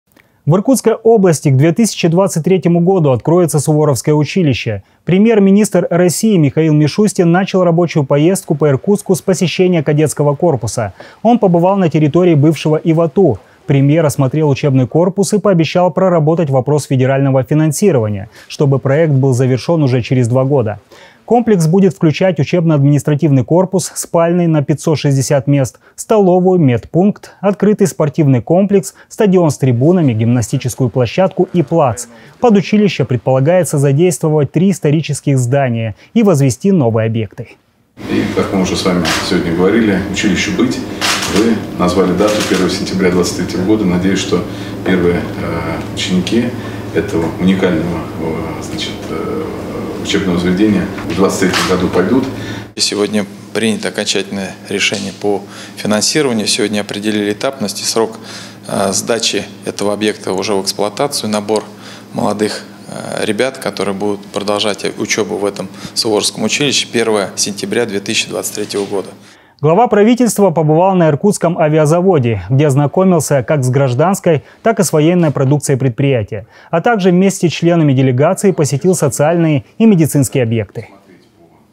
В Иркутской области к 2023 году откроется Суворовское училище
Премьер-министр России Михаил Мишустин начал рабочую поездку по